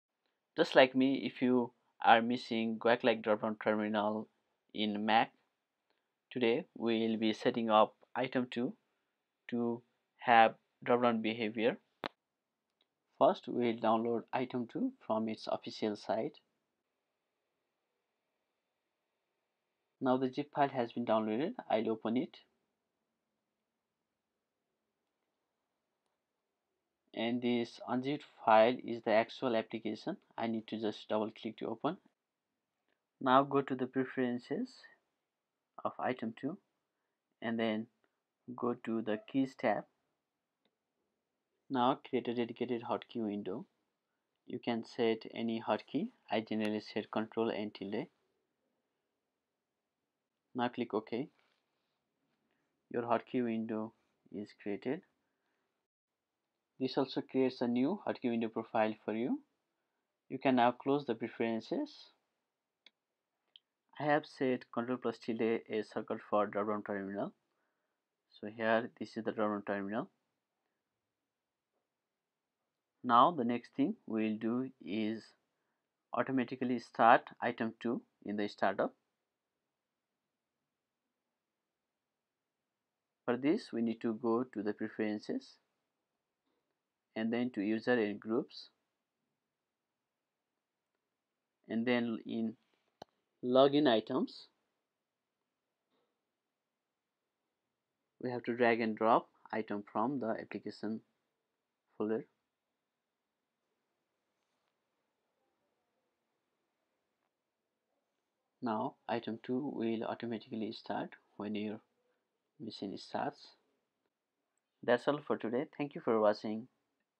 Guake like dropdown terminal in mac os with iterm2
How to configure iterm2 to have guake like dropdown terminal in mac
iterm version: 3.1.5